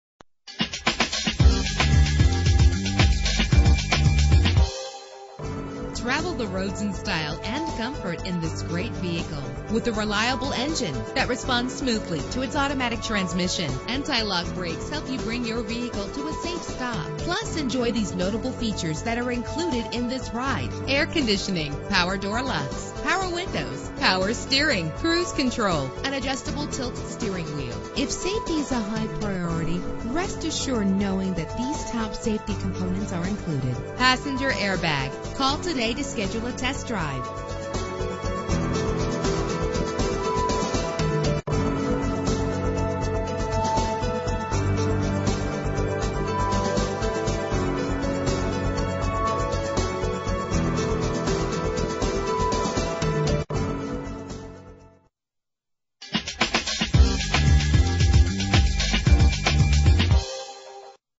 2007 Ford F150 SuperCrew Cab - Houston TX
Year: 2007
Make: Ford
Model: F150 SuperCrew Cab
Trim: XLT Pickup 4D 5 1/2 ft
Engine: 8 cylinder 5.4 liter
Transmission:
Color: Red
Mileage: 89619
Address:
12333 Gulf Freeway